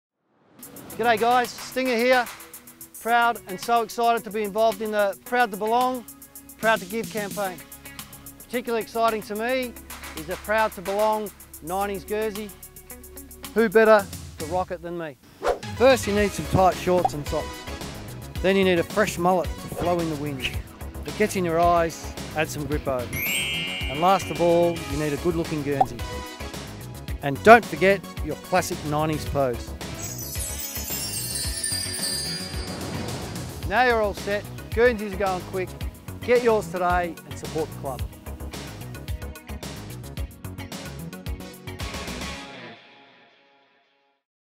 Proud to Belong | 90's Guernsey
Former Melbourne great Stephen Tingay shows us how to rock the new 'Proud to Belong 90's Guernsey'.